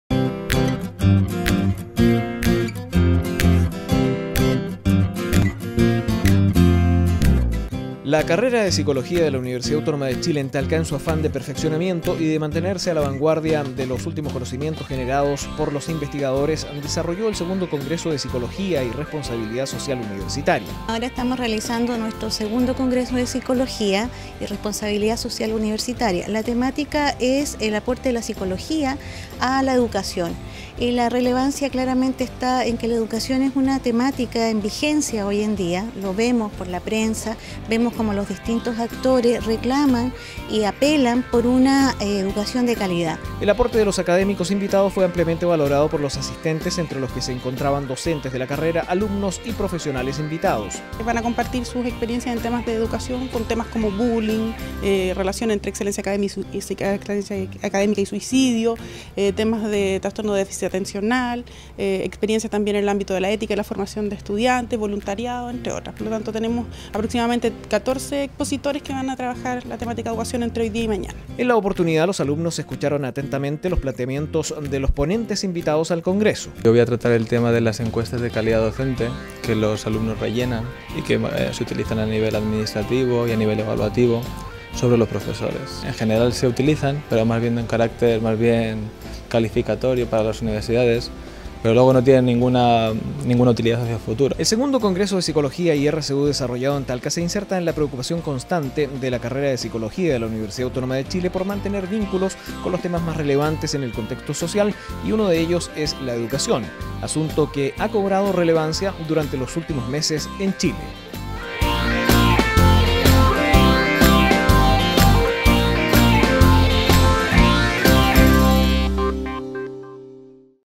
II Congreso de Psicología - Universidad Autónoma de Chile, Talca
La Universidad Autónoma de Chile en Talca, realizó el “II Congreso de Psicología y Responsabilidad Social Universitaria”. La actividad fue organizada por la carrera de Psicología en Talca y contó con la participación de ponentes locales y de otras universidades, quienes abordaron tópicos referentes al aporte de la psicología a la educación.